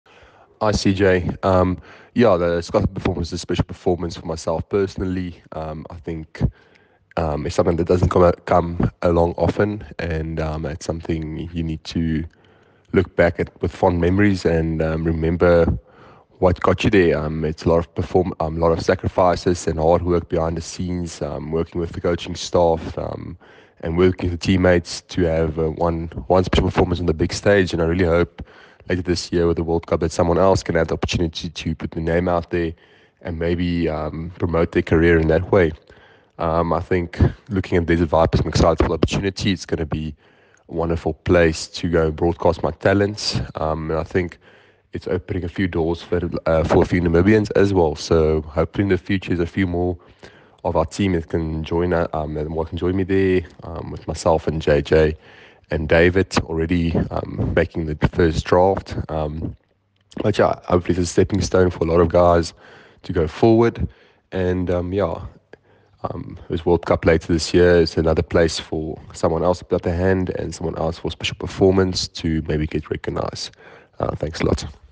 Ruben Trumpelmann
Namibian pace bowler Ruben Trumpelmann on being selected for the Desert Vipers that will compete in the UAE International T20 League. Video: Helge Schütz